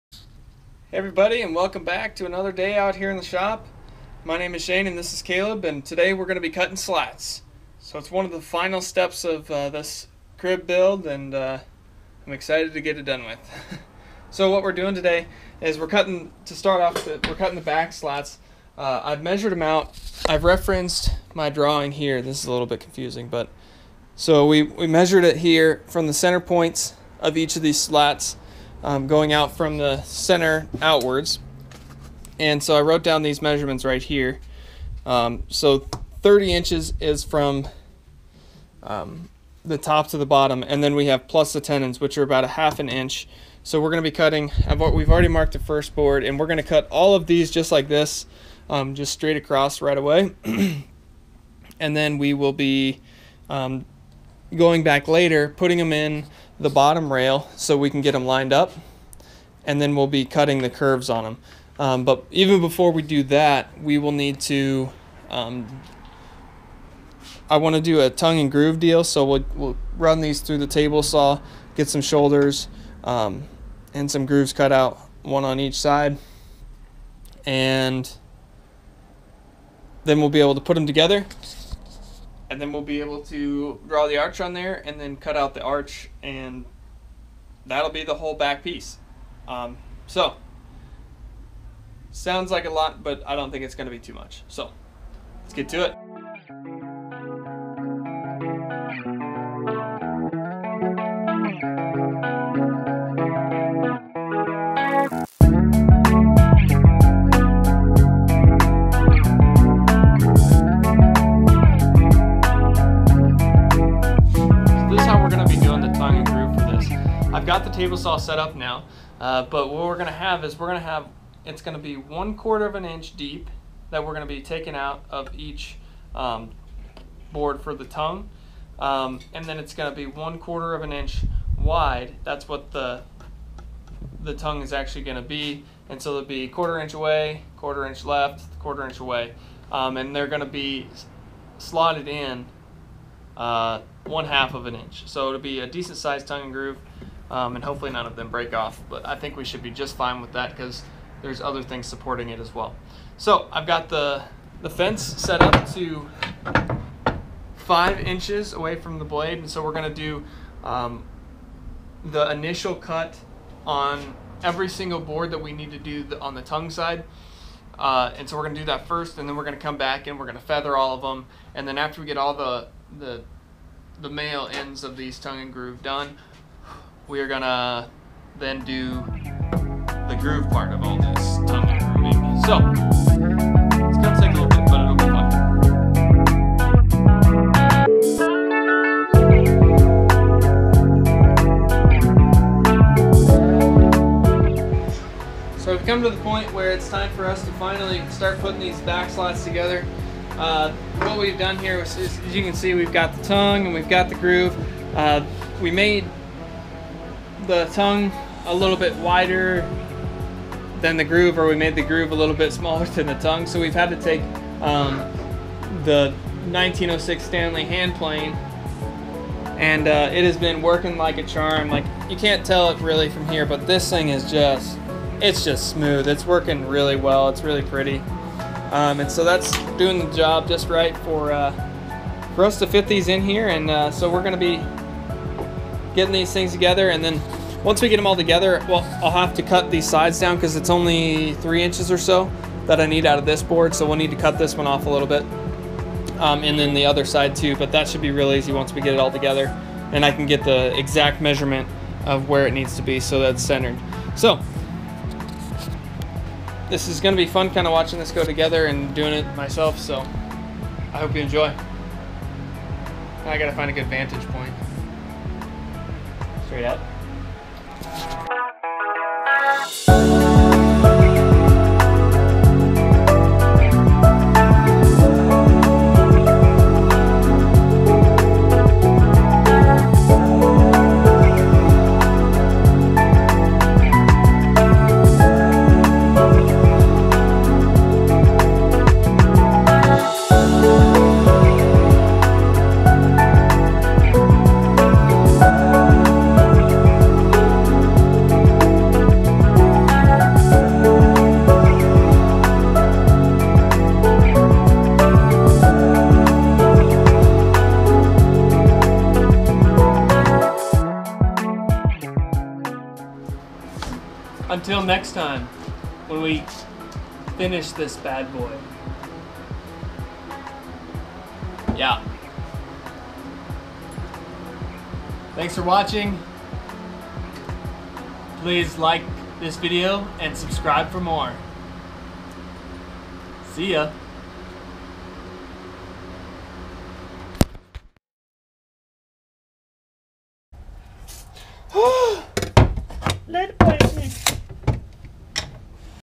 Baby Crib Build Part 12: Back Slats
Today on the Outdoor Tinkerer, Caleb and I do a ton of cutting to finish up the slats for the backing of the crib. The slats are made of hickory and the frame is made from red cedar. We are so close!